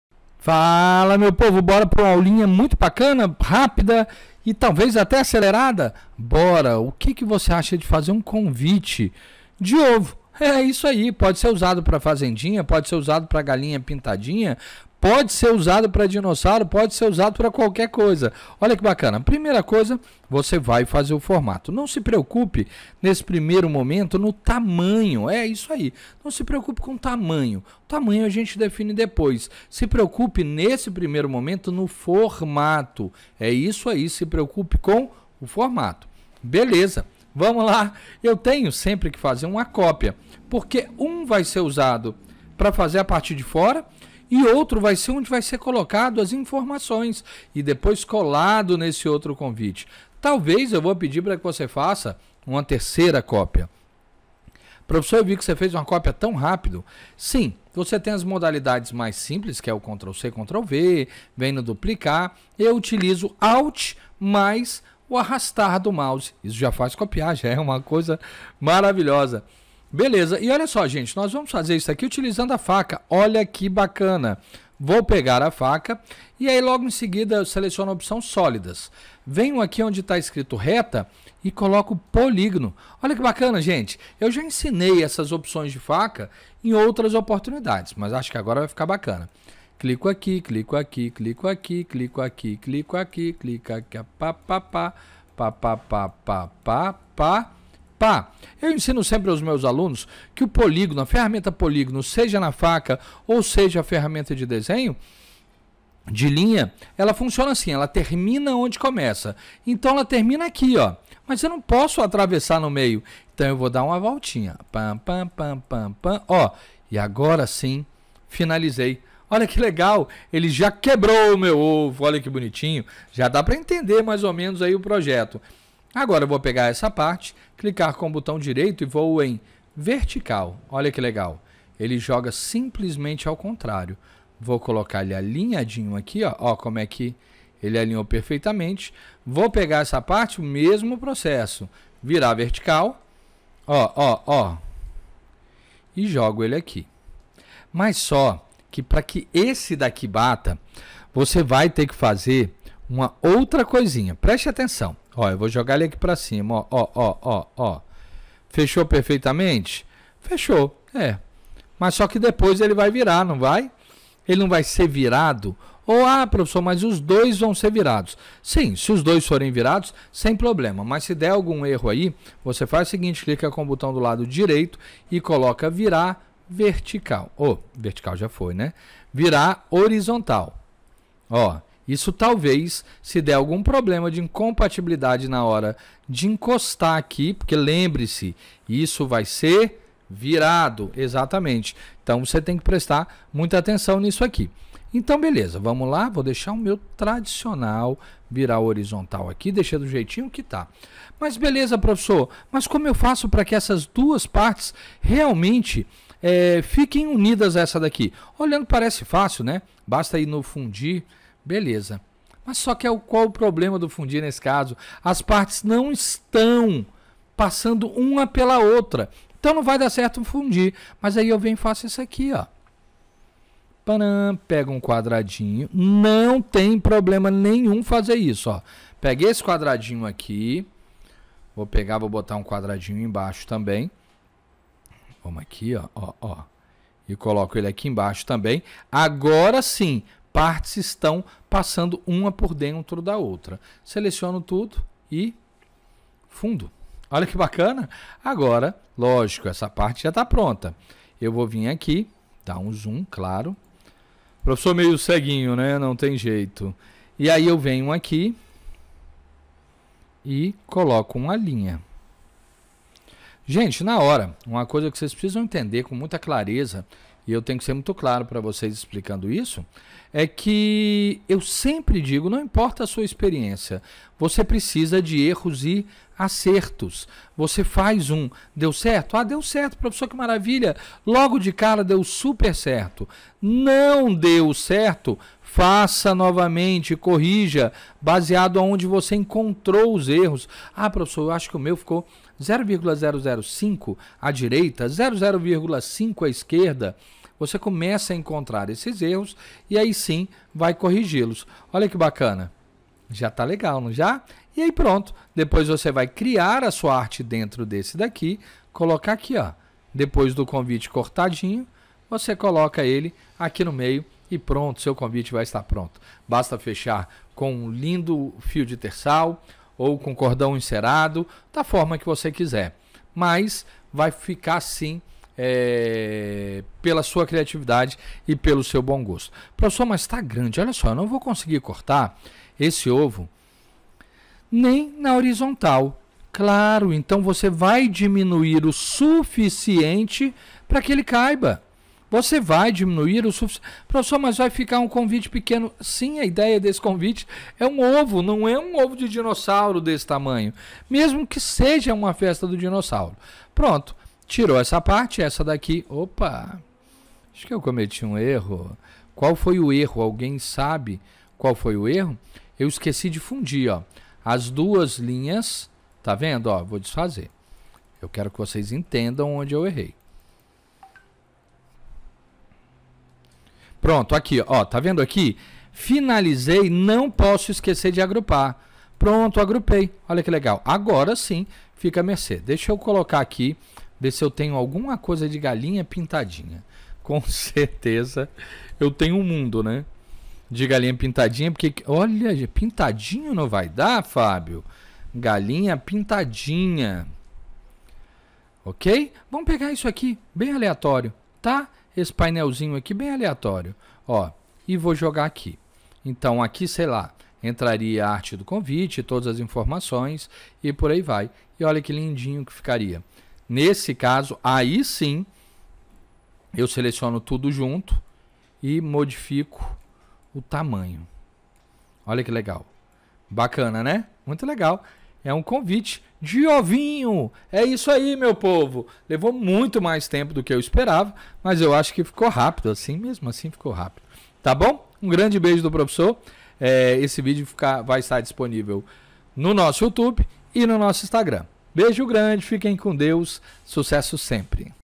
Desenhando um Convite de Ovinho.
Digo sempre que tudo se torna facil quando sabemos né. rsrsrs.

Então aprenda a desenhar esse lindo molde de CONVITE no formato de  OVINHO!